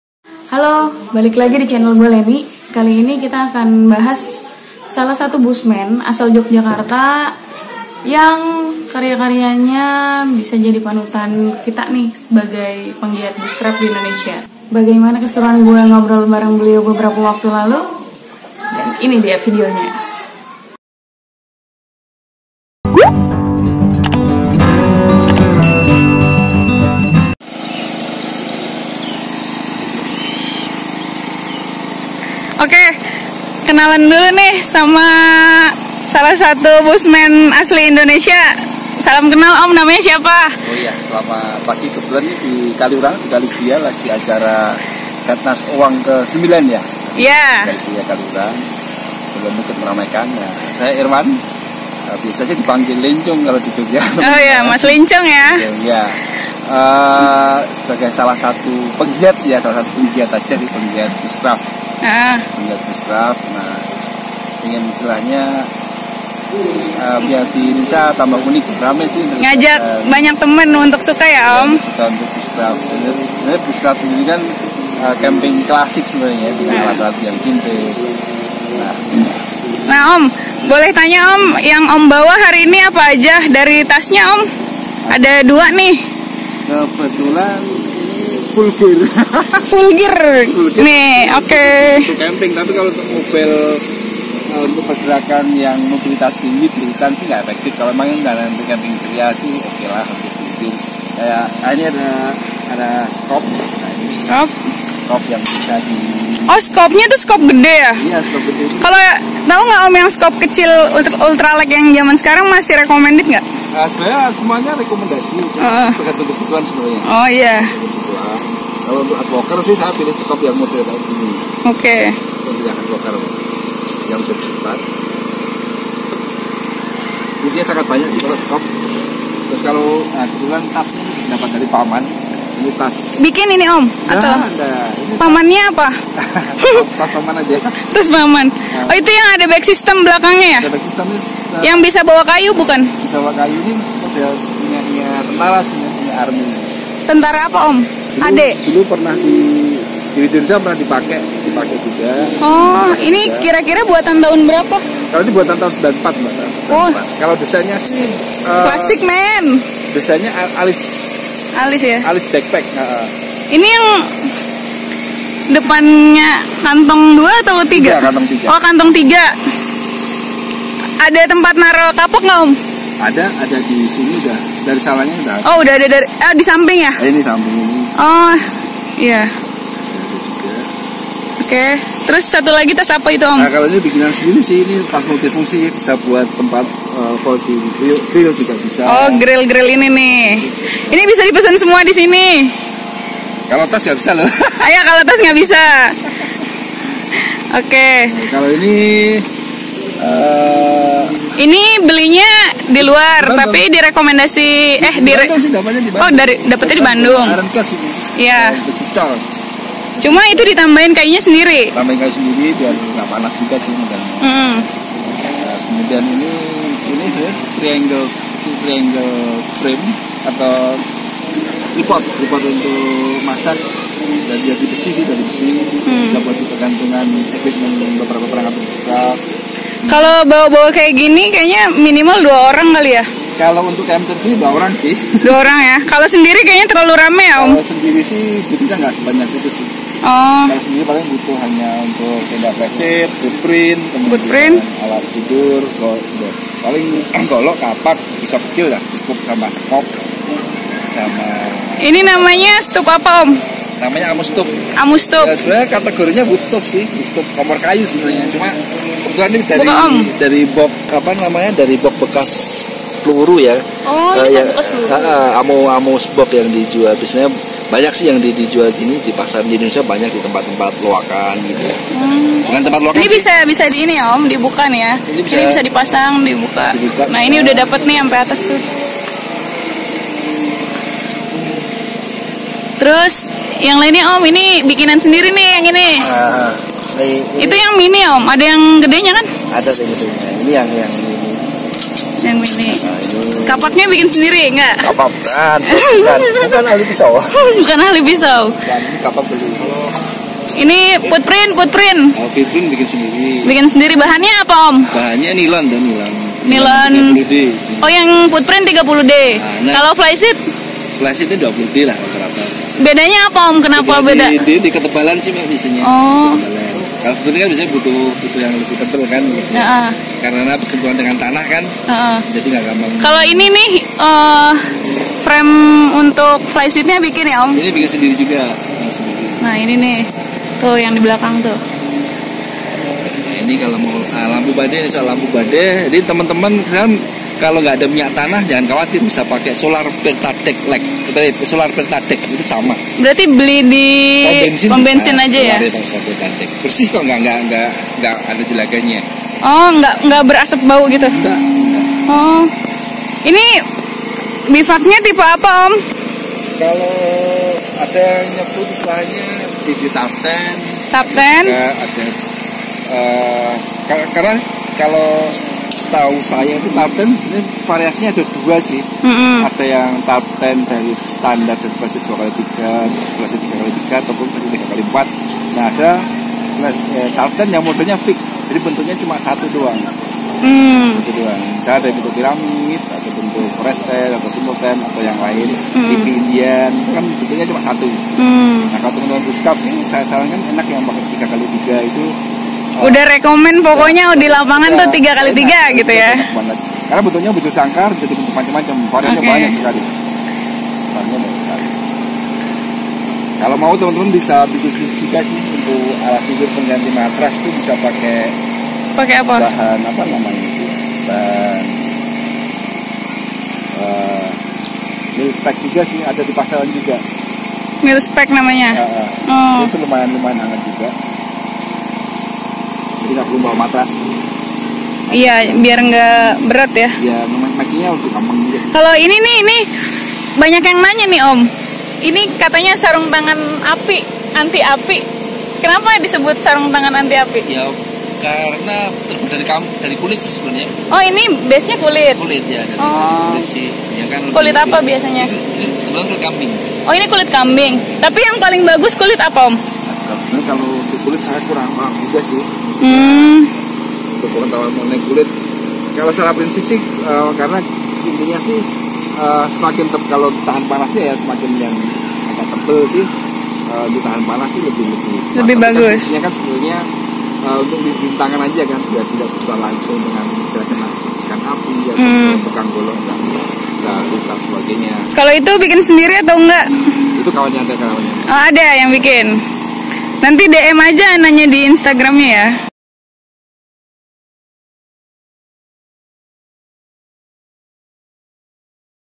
BUSHCRAFT INDONESIA (Full Gear Bushman) II REVIEW PERALATAN BUSHCRAFT LENGKAP
Buat yang kemaren minta video review flysheet lanjutan, maaf yah. belum sempet.
Di video ini akan membahas full gear Bushman dari Jogja yang biasa digunAkan untuk kemah (gunung).

dan informan didalam konten ini @the_woodstockgear
Saya akan sangat senang apabila kalian memberikan komentar pada video ini.